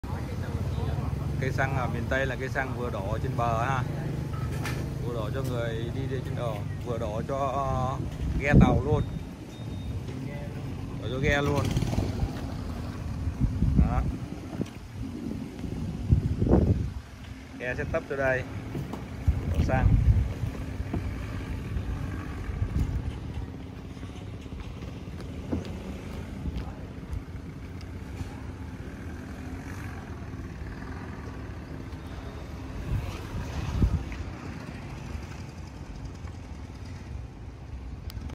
Độc đáo của cây xăng ở Miền Tây, vừa cho xe vừa cho ghe thuyền
Đặc trưng của sông nước Miền Tây là cứ bên lộ sẽ cặp kè là sông. Thực chất là kênh rạch được đào bởi sức người. Moi đất lên làm đường thì để lại là con kênh. Vì thế thuận lợi cả đường bộ và đường thủy.
Nếu ai đi trên máy bay, ngang đồng bằng Sông Cửu Long sẽ thấy những con kênh dài thẳng tắp. Thẳng như chính tấm lòng con người Nam Bộ. Kênh đào và đường cặp bên được thể hiện rất rõ qua tác phẩm điện ảnh Ngọc Trong Đá. Ai từng xem phim do Lê Công Tuấn Anh đóng và đọc tác phẩm văn học gốc, chuyển thể thành phim sẽ thấy được lịch sử hình thành hệ thống kênh rạch chằng chịt. Và bạn sẽ cảm nhận được chất Nam Bộ nhiều hơn.
Cây xăng cũng được hình thành trên cơ sở đặc thù của miền đất sông nước.